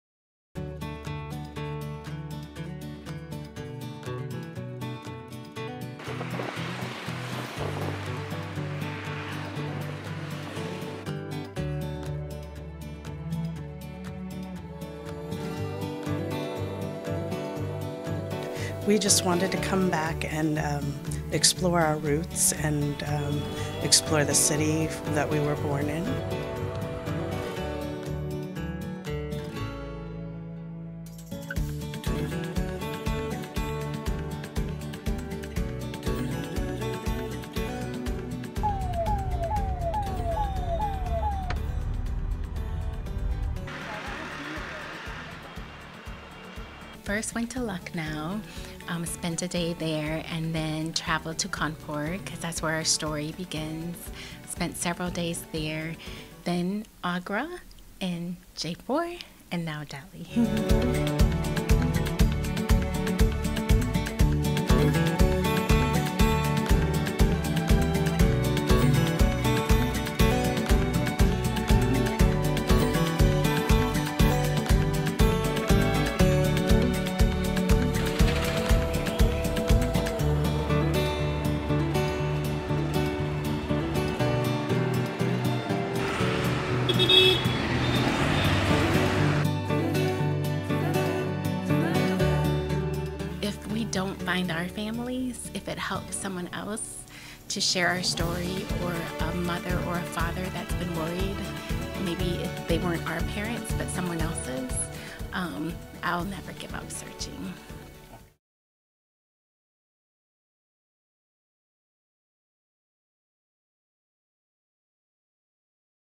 Two Women Searching Their Roots In India (BBC Hindi)
कानपुर के एक अनाथालय से 40 साल पहले अमरीका पहुंचीं दो महिलाएं अब अपने असली मां-बाप को ढूंढ़ने निकली हैं. उन्हें दो अलग-अलग अमरीकी परिवारों ने गोद लिया था. अमरीका में वो दोनों 'लॉस्ट सारीज' नाम का एक संगठन भी चलाती हैं जो उनके जैसे गोद लिए जाने वाले लोगों की मदद करता है.

वीडियो: ब्रजेश मिश्र